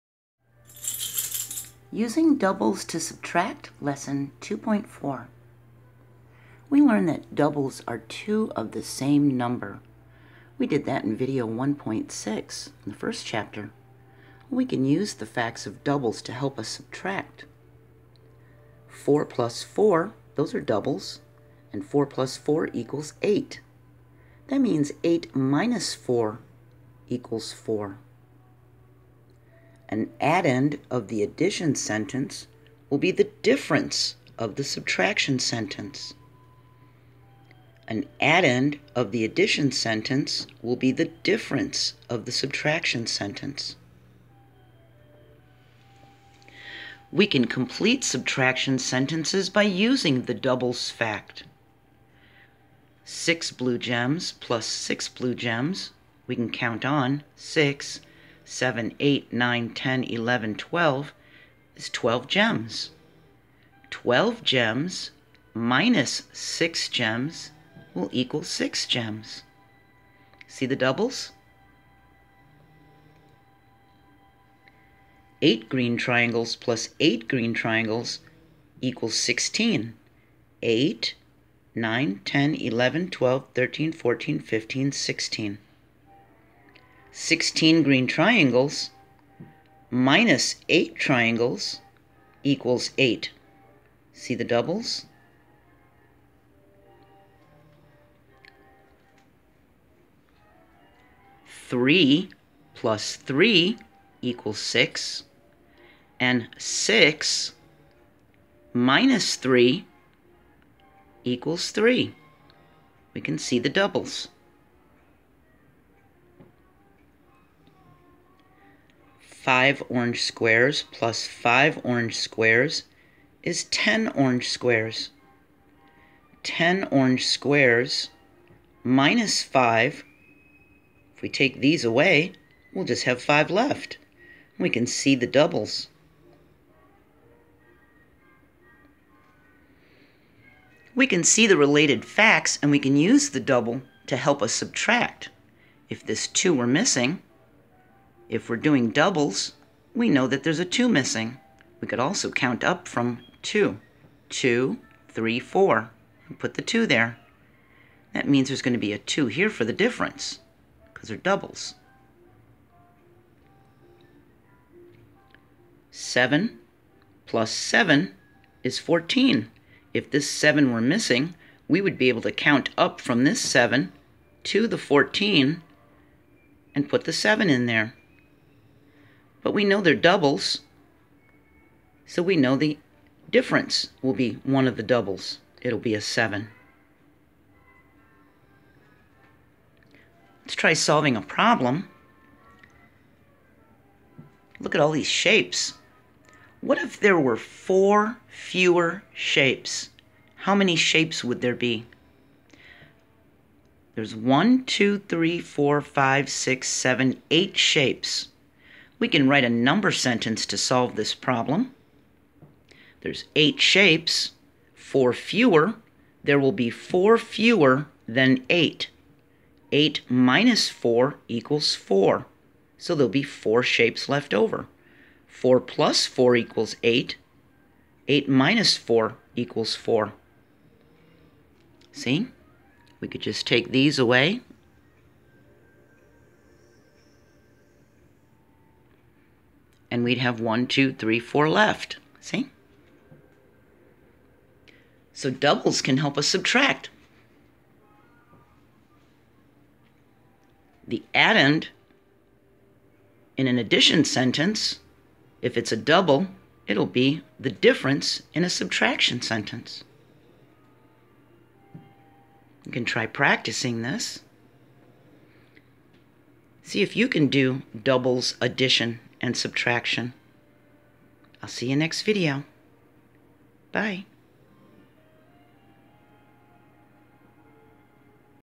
Grade 2 Math 2.4, Using doubles to subtract
How to use the facts of doubles to help us subtract. Page 41 in the Silver Burdett Grade 2 textbook.